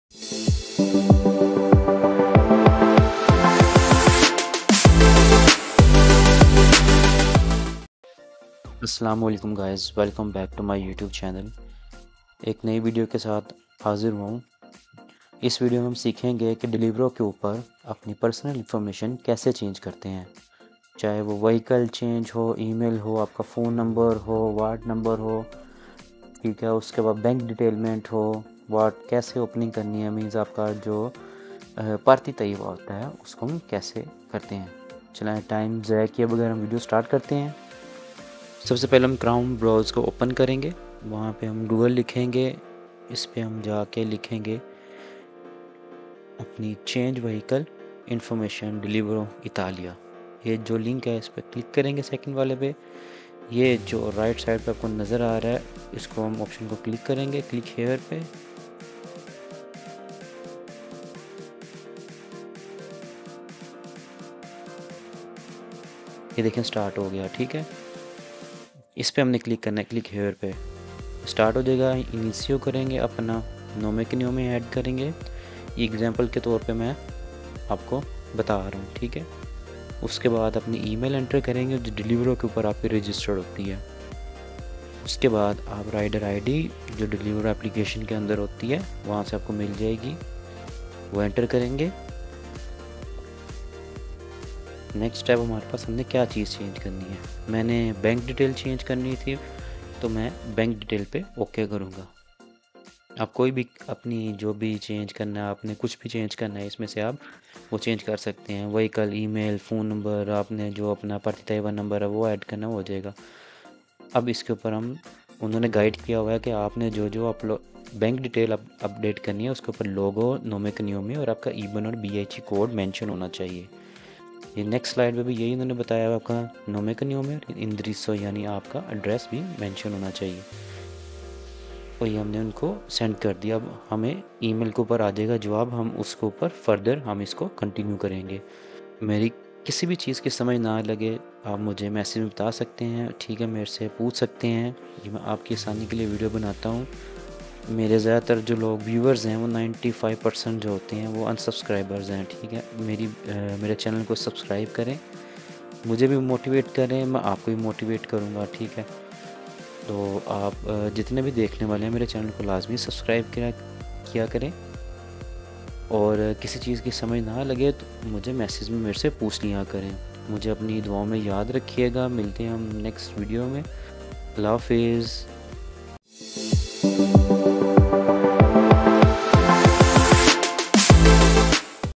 How to Change Your Name, Email & Address in Deliveroo App (Step-by-Step Guide)
Whether you're a customer or a rider, this step-by-step guide will help you make changes easily.

🔹 Update Name
🔹 Change Email
🔹 Edit Delivery Address